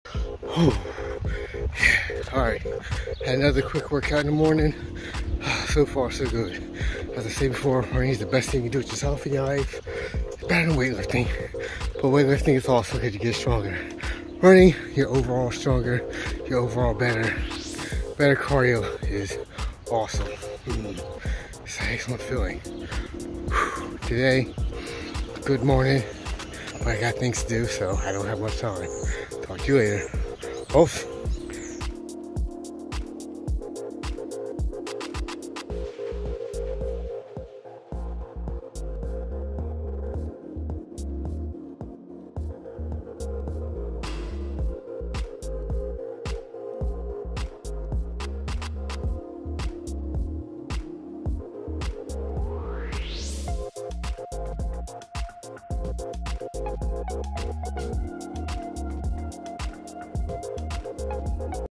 A Great Morning For Running!
I got outside forba good moring run. Today was perfect and I'm happy to train.

Get Up And Run!

Men's Pop Art Yoga Pants - Roy Lichtenstein Inspired Dahalia Print 001c